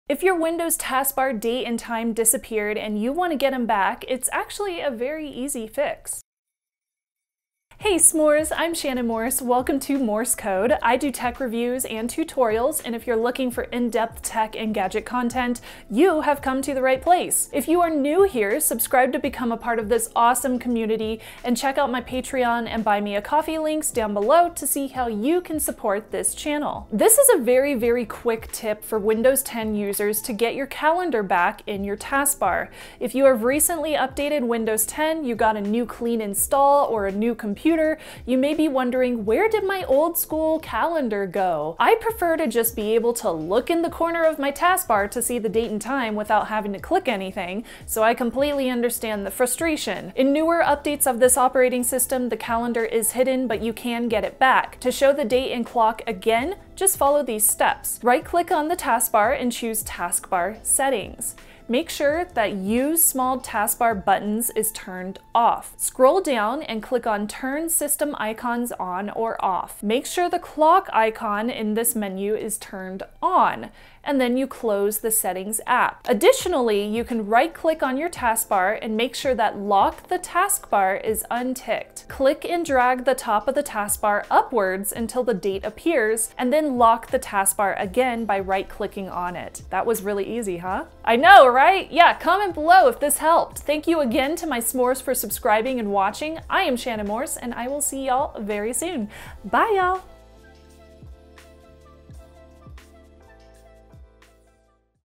How To Show Date, Time Clock, and Day Of The Week On Your Windows 10 Taskbar \ Morse Code Quick Tips
Taskbar clock disappeared?
If your windows taskbar date or time disappeared and you wanna get 'em back, it's a really easy fix.

Mail ✈
SHANNON MORSE
558 CASTLE PINES PKWY UNIT B-4 #198
CASTLE PINES CO 80108
UNITED STATES

Music from Pond5 🎵 and YouTube's Music Library